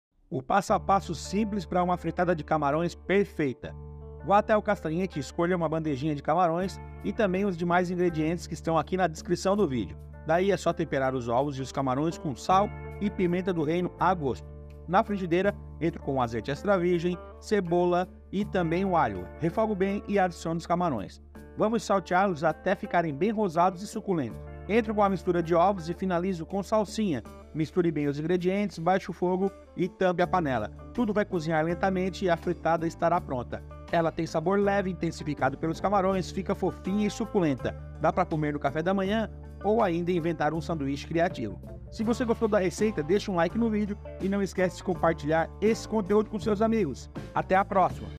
FRITADA DE CAMARÕES | frittata di gamberetti
Que sair da rotina? Então passa no Castagneti, compra uma bandejinha de camarão e bóra fazer essa frita de camarões deliciosa. A frittata di gamberetti proporciona um sabor adocicado, delicioso, que contrasta com a crocância dos ingredientes do refogado.

Se você é um entusiasta da culinária que busca por pratos que destacam o melhor dos ingredientes frescos, a nossa fritada de camarões e salsa é a escolha ideal. Nossa receita de salsa fresca proporciona um toque revitalizante, complementando perfeitamente a intensidade do camarão. Com ingredientes simples e um processo descomplicado, esta receita de

E-MAIL PARA DÚVIDAS
NÃO CLIE AQUI!

Ingredientes:

250g de camarões médios, descascados e limpos
4 ovos
1/2 xícara de salsa fresca picada
1/4 de cebola roxa picada
1 dente de alho picado
2 colheres de sopa de azeite de oliva